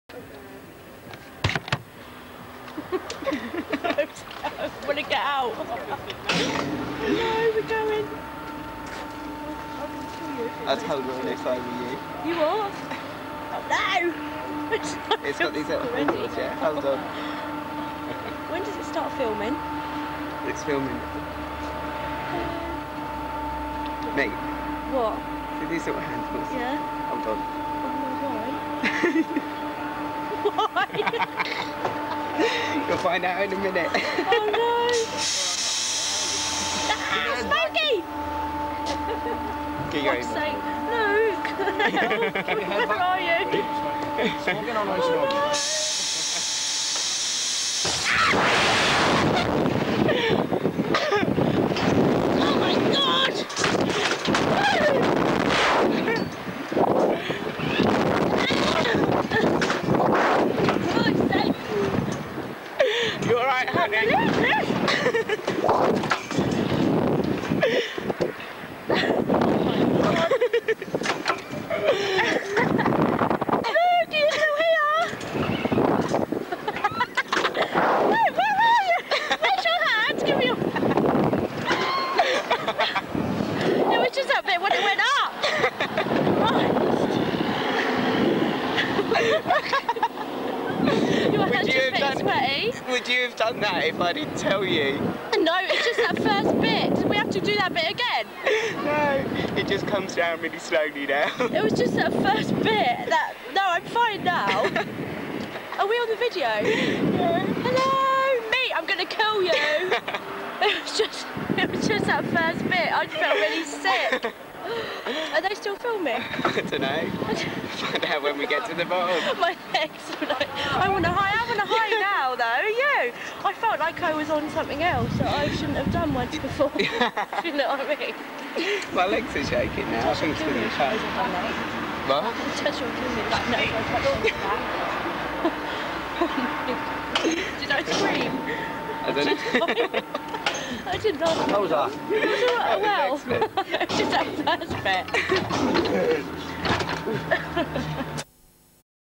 Bungy Rocket
Back in the summer of 1999 in san antonio Ibiza I took a girl called Sarah to the bungy rocket.  Being very carefull for her not to see what happened on it i got her in to the hot seat.  And this is what happened. Ain't I a stinker!